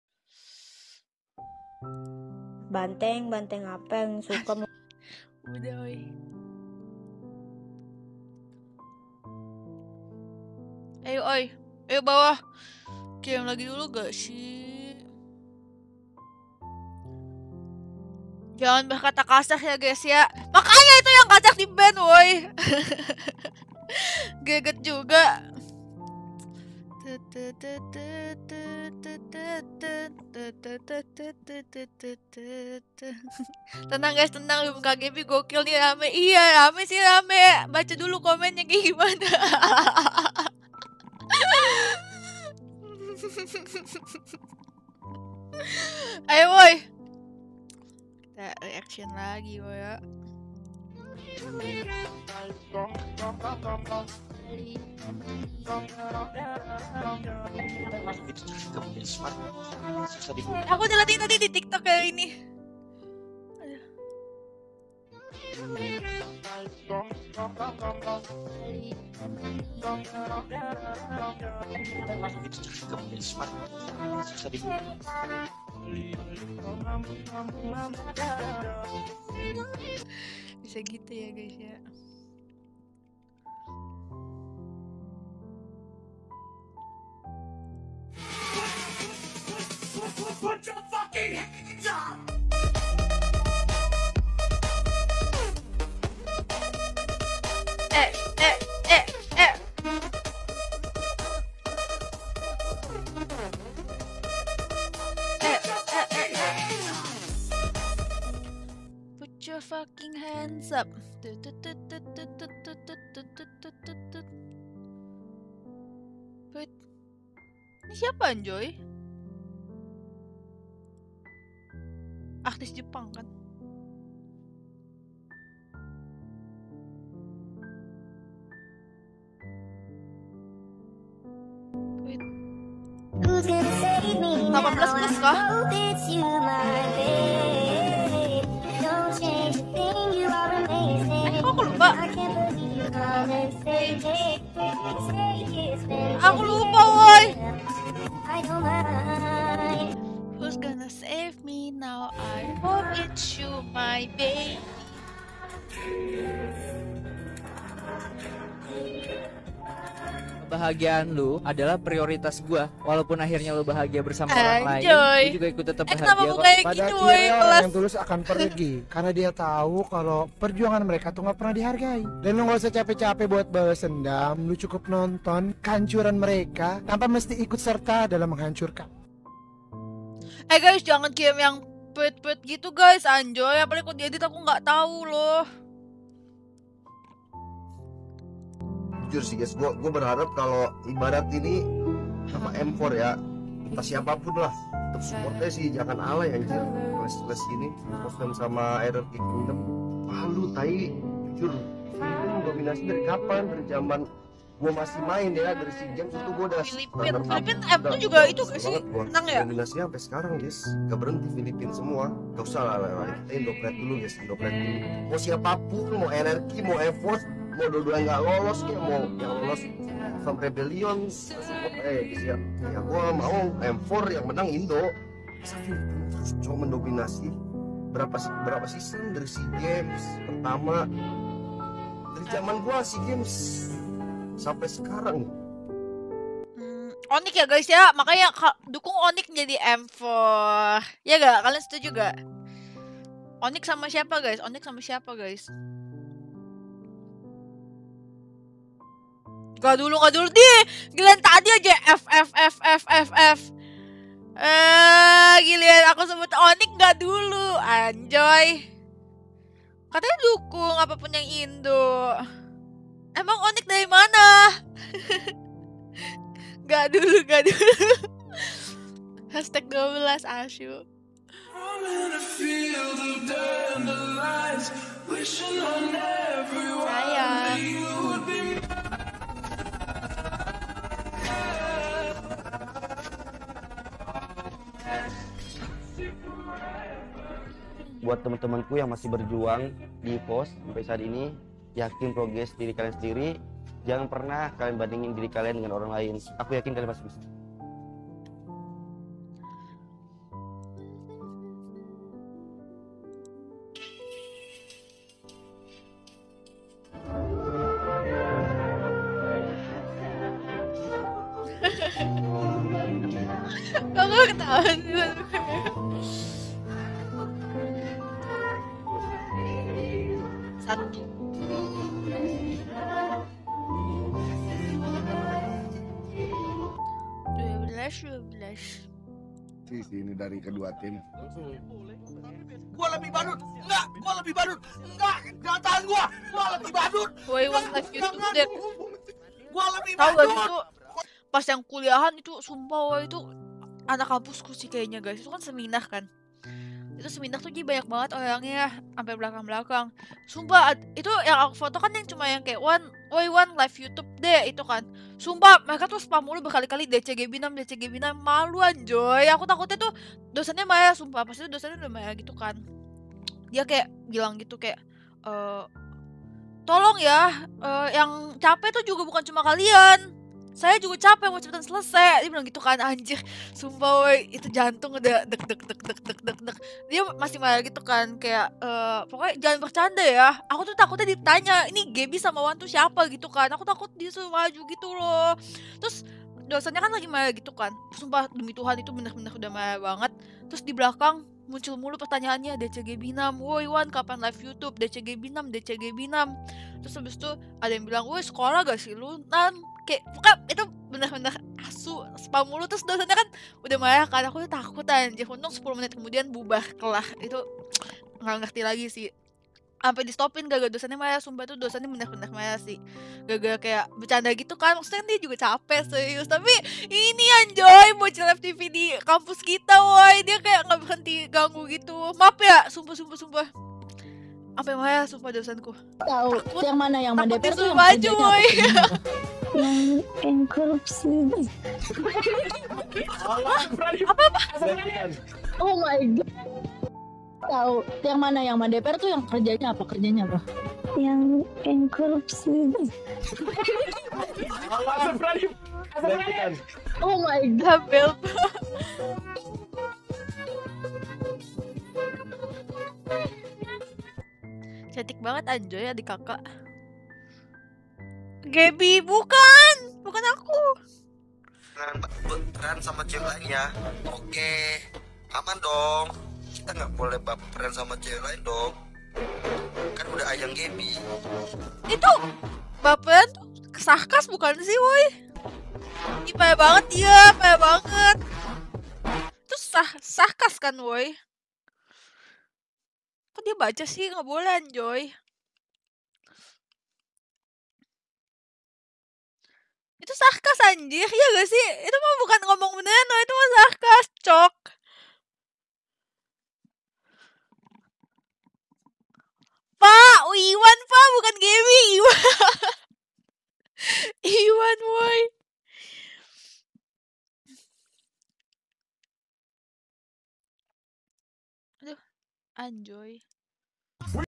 Reaksi GEBI Ketika WANN Bilang Gak Boleh Baperan Sama Cewek Lain - GEBI Curhat Soal Spam DC Gebinam
REAKSI GEBI KETIKA WANN BILANG GAK BOLEH BAPERAN SAMA CEWEK LAIN
REAKSI GEBI DENGAR WANN BILANG GEBI CAKEP TAPI SUSAH DIMILIKI
GEBI CURHAT DOSEN MARAH KARENA SPAM DC GEBINAM SAAT SEMINAR